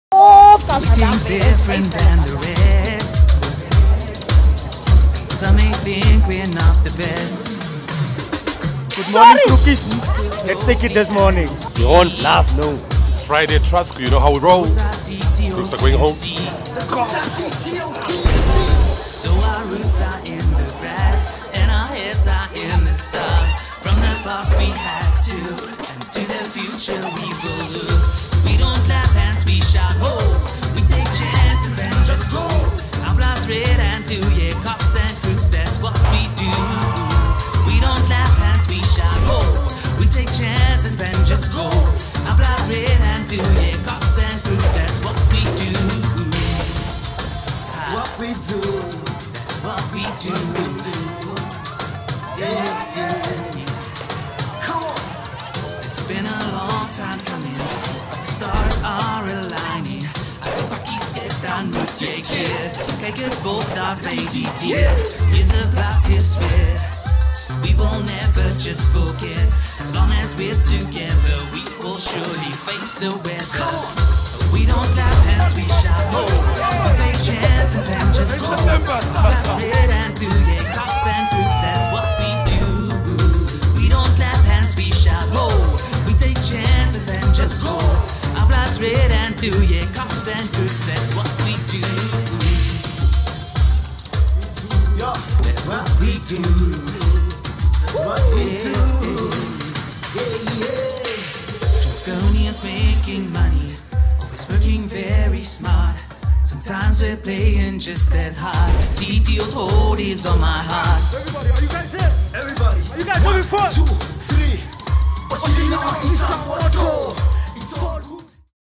Cops find their balloon popped!
The two warring teams of Trustco's weekly Friday morning social games clashed today in icy wintry weather. Luckily, all the smashing and thrashing of balloons warmed them up quickly, with the Trustco Crooks finding the heat with a win as cold muscles hampered the Cops. With their hopes of a winning streak bursting in their faces, the Cops found their ambitions chilled, while the Crooks are now looking forward to an afternoon in the sun.
At Trustco Ongwediva, the fortunes were reversed as the Cops won their bouts in both flip the bottle as well as orange hoop hits.
Cops, lekker werk! Enjoy your half-day in the sun, Crooks. Lekker naweek Trustconians! HOU!!!
Overall score for the day: Cops 3, Crooks 4